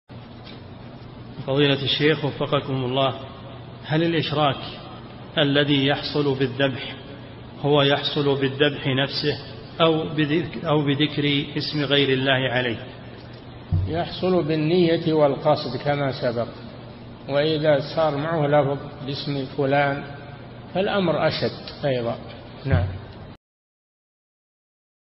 [842 -939] هل الإشراك الذي يحصل بالذبح هو يحصل بالذبح نفسه، أو بذكر اسم غير الله عليه؟
-[842 -939] السؤال: هل الإشراك الذي يحصل بالذبح هو يحصل بالذبح نفسه، أو بذكر اسم غير الله عليه؟
الجواب: يحصل بالنية والقصد كما سبق، وإذا صار معه لفظ باسم فلان فالأمر أشد أيضا.
للاشتراك في قناة الشيخ صالح الفوزان

للاستفسار والإقتراحات والدعم
رئيس الفريق
لمشروع كبار العلماء